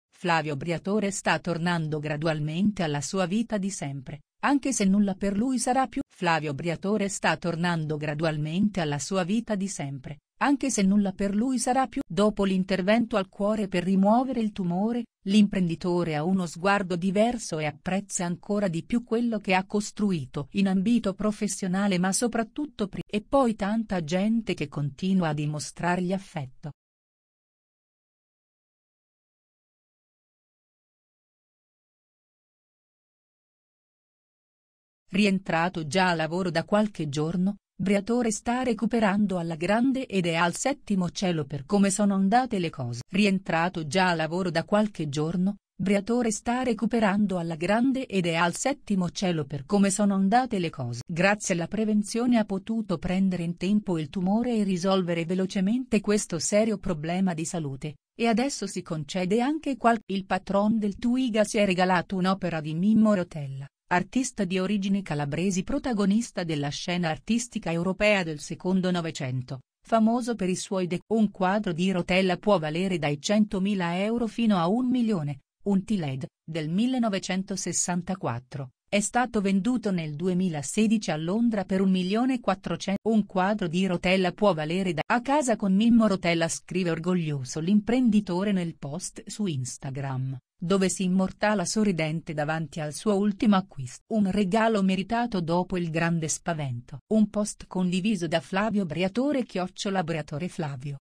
Briatore torna a casa dopo il tumore e si fa un regalo da centinaia di migliaia di euro
Briatore torna a casa dopo il tumore e si fa un regalo da centinaia di migliaia di euro Flavio Briatore sta tornando gradualmente alla sua vita di sempre, anche se nulla per lui sarà più come prima. Dopo l'intervento al cuore per rimuovere il tumore, l'imprenditore ha uno sguardo diverso e apprezza ancora di più quello che ha costruito in ambito professionale ma soprattutto privato. Accanto a lui l'ex compagna Elisabetta Gregoraci - che non lo ha mai lasciato solo durante la degenza in ospedale - e il figlio Nathan Falco, ma anche gli amici e i collaboratori più fidati. E poi tanta gente che continua a dimostrargli affetto. Rientrato già al lavoro da qualche giorno, Briatore sta recuperando alla grande ed è al settimo cielo per come sono andate le cose. Grazie alla prevenzione ha potuto prendere in tempo il tumore e risolvere velocemente questo serio problema di salute, e adesso si concede anche qualche coccola. Il patron del Twiga si è regalato un'opera di Mimmo Rotella, artista di